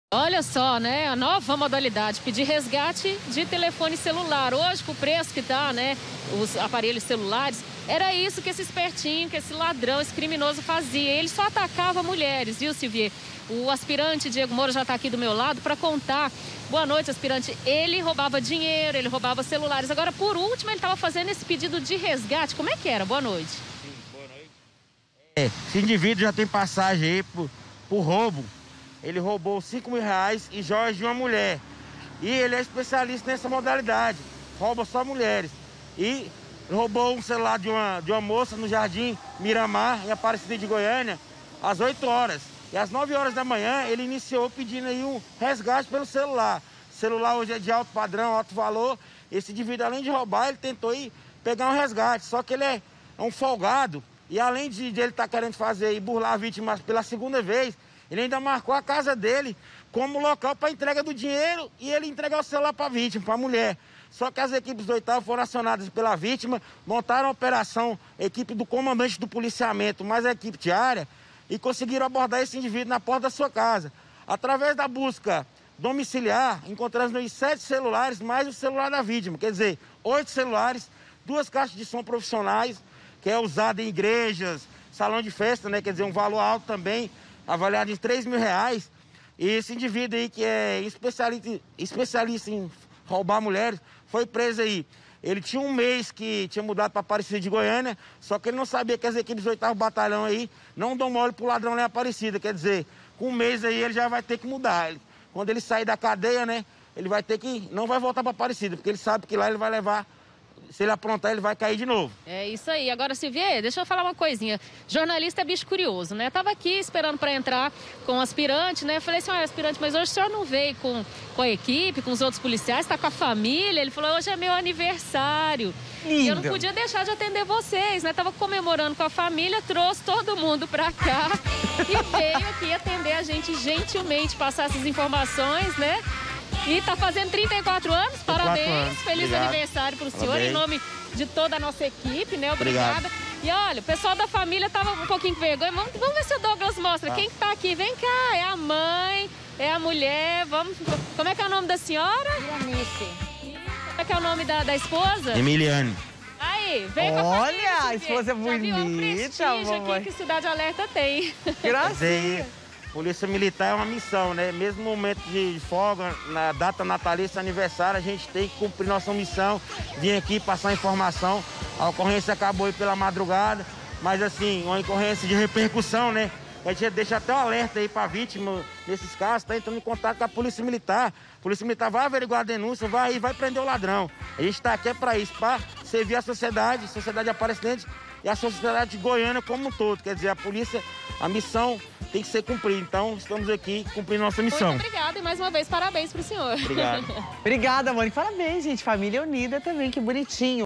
SEQUESTRADOR DE CELULAR HOMEM ROUBAVA E PEDIA RESGATE PARA VÍTIMAS
UM LADRÃO ROUBAVA CELULARES E DEPOIS LIGAVA PARA PEDIR O RESGATE DO BEM PARA A PRÓPRIA VÍTIMA.